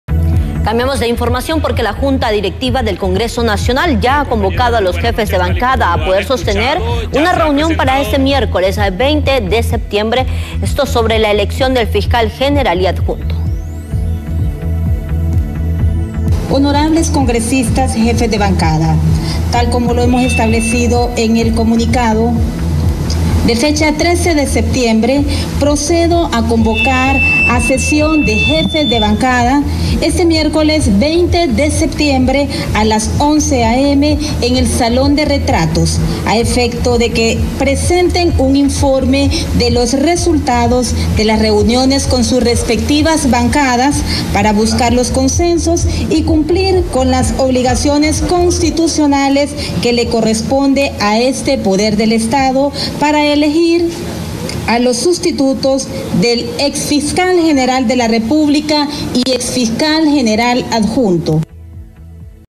Convocan a los jefes de bancada para elegir a los fiscales
Los diputados se reunirán el miércoles 20 de septiembre para presentar un informe, por bancada, y hallar un consenso sobre la elección.
La reunión está programada para las 11 a. m. en el Salón de Retratos.